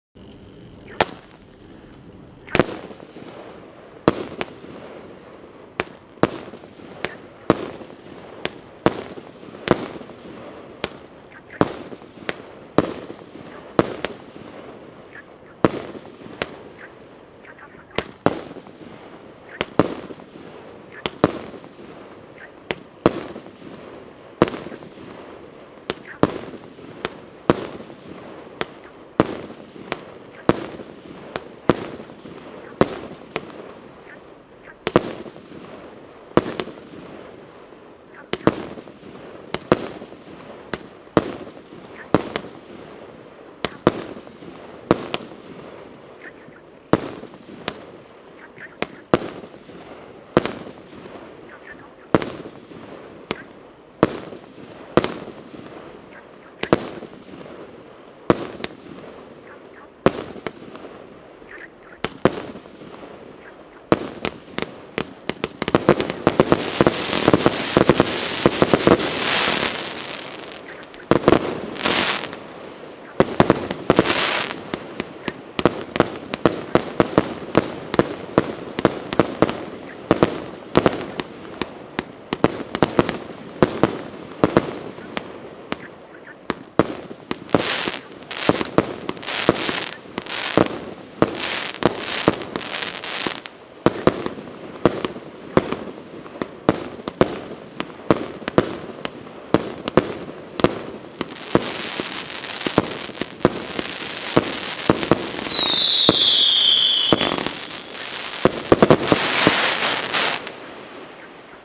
БСК0115808 12 ОЛИМП
Фейерверк, батарея салютов Олимп 158 х 0,8"-1,2" купить
или в розничном магазине салютов ВесЛандия рядом с м. Коньково.
Адрес: Коньково, Москва, Профсоюзная улица, 109 (2 этаж, магазин ВесЛандия).